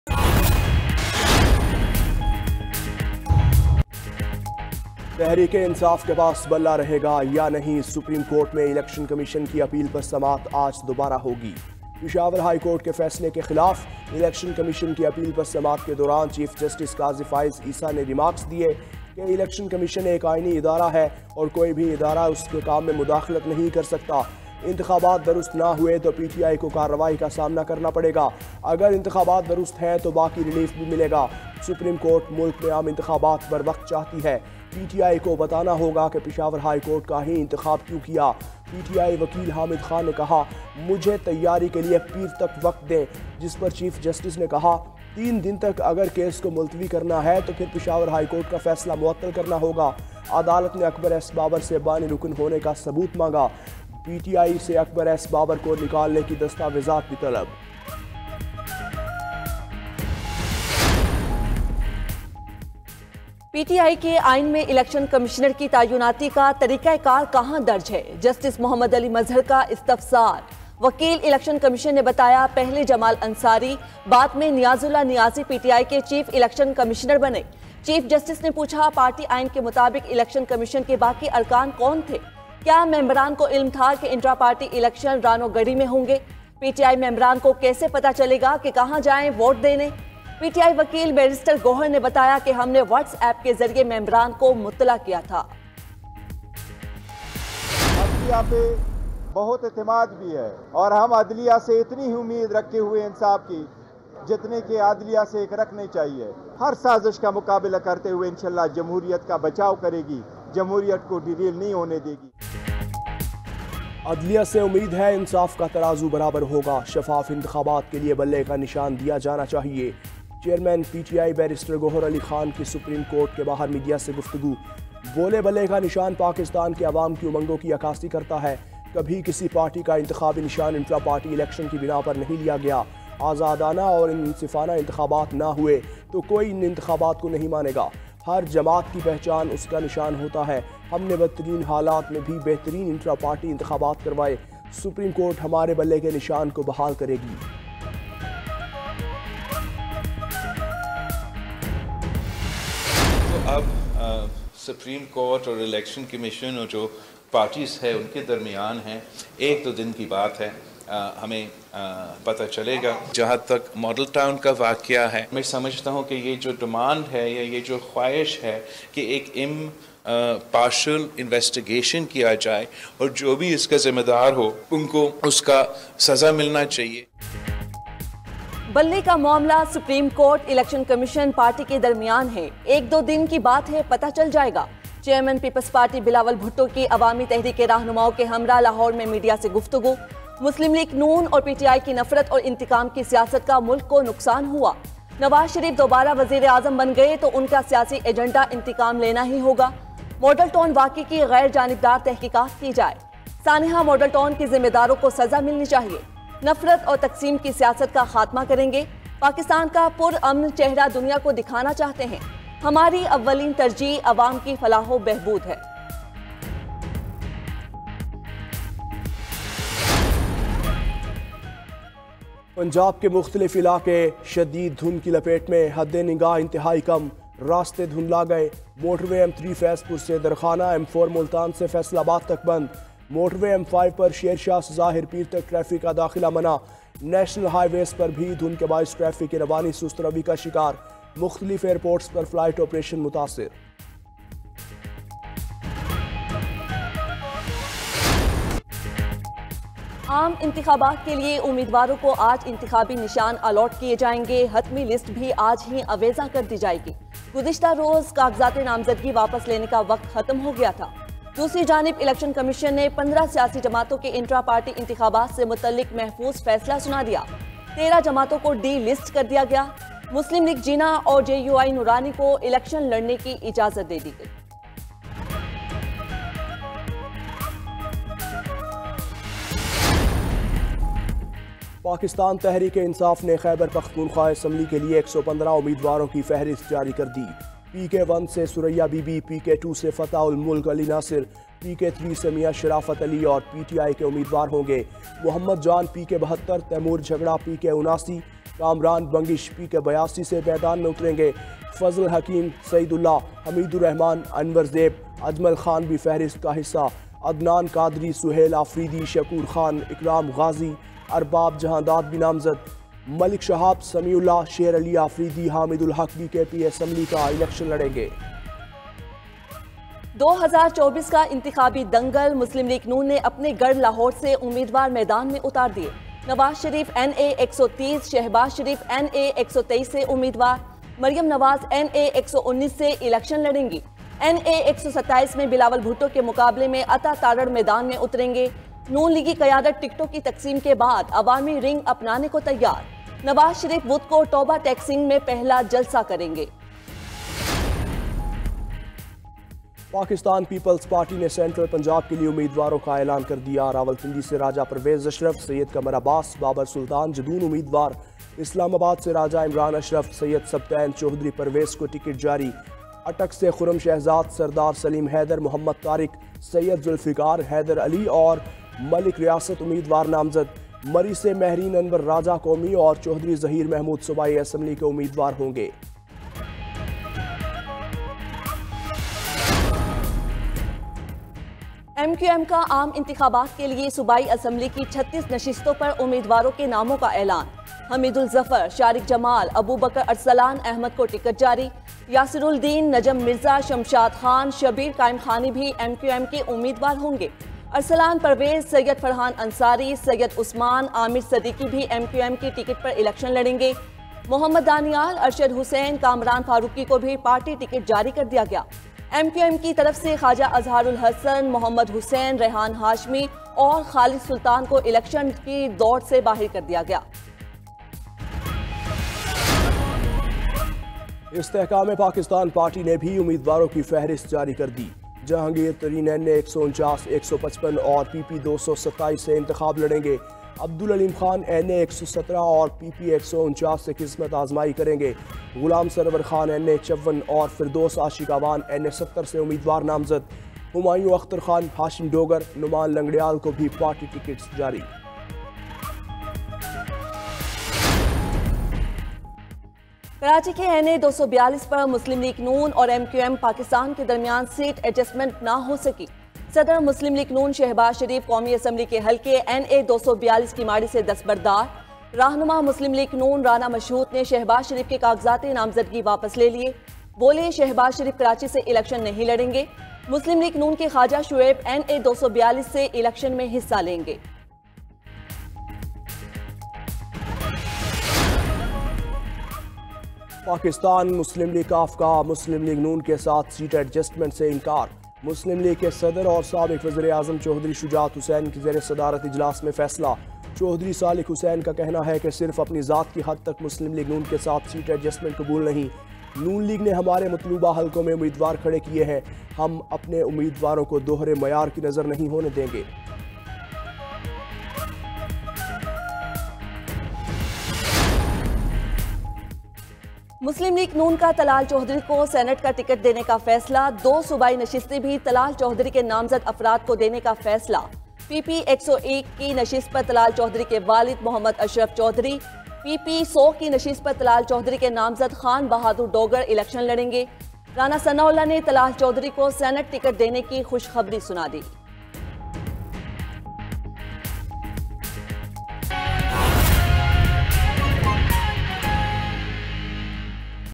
92 News Headlines 9 AM | Qazi Faez in Action | Historic Verdict on PTI Bat Symbol | 13 Jan 2024
92 News Headlines 9 AM | Qazi Faez in Action | Historic Verdict on PTI Bat Symbol | 13 Jan 2024